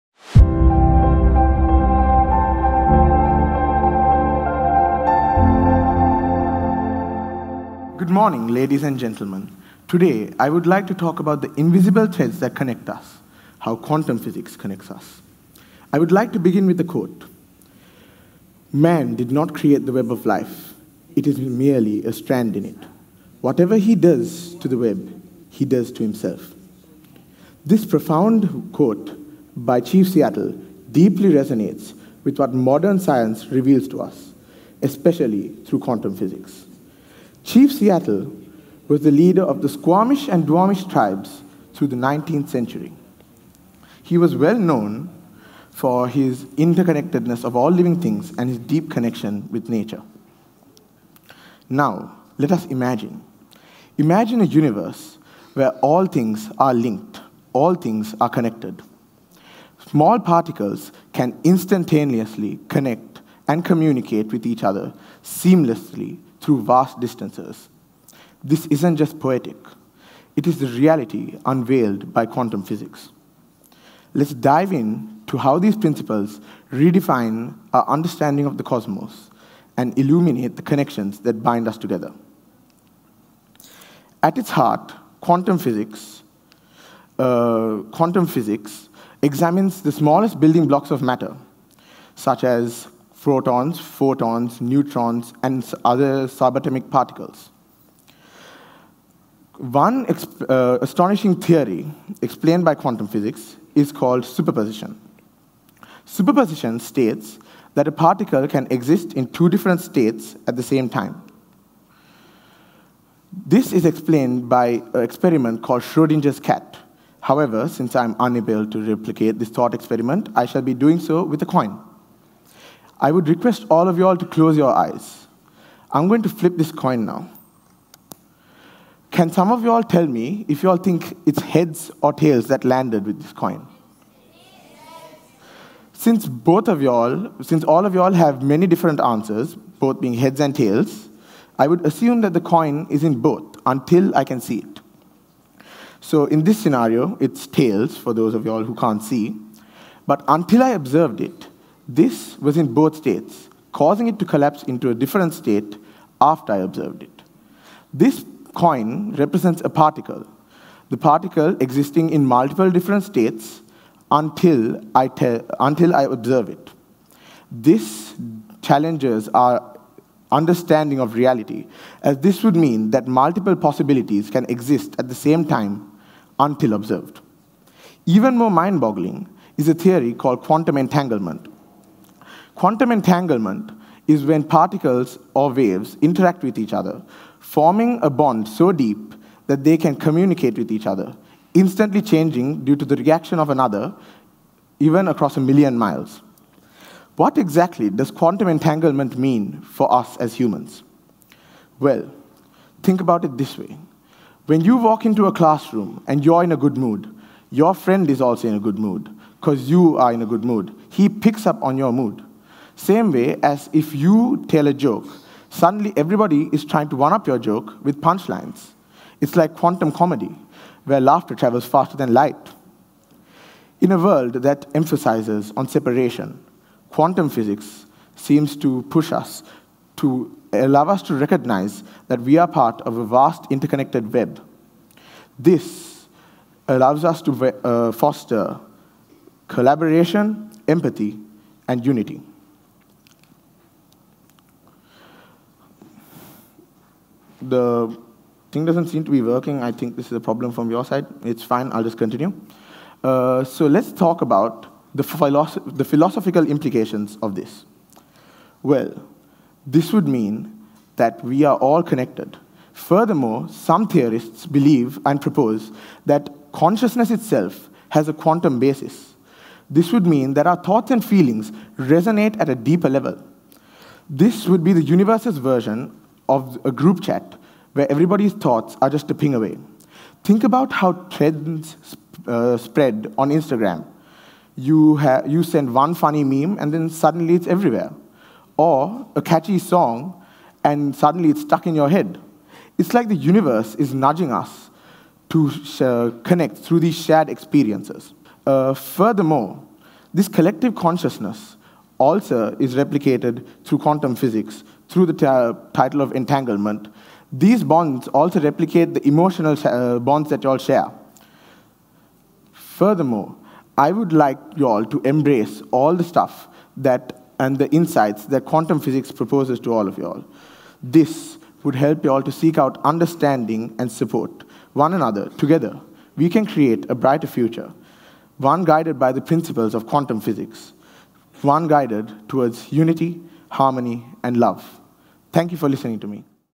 The Invisible Threads: How Quantum Physics Connects Us All | Jason Anthony | TEDxYouth@GCNegombo
Exploring the concept of interconnectedness through quantum physics, this speech by Jason Anthony from Gateway College Negombo explains how principles like superposition and entanglement reveal our deep connections to the universe and one another. Drawing parallels between quantum phenomena and human relationships, the speaker emphasizes the importance of empathy and collective consciousness in fostering unity.  Youth speaker at TEDxYouth@GCNegombo This talk was given at a TEDx event using the TED conference format but independently organized by a local community.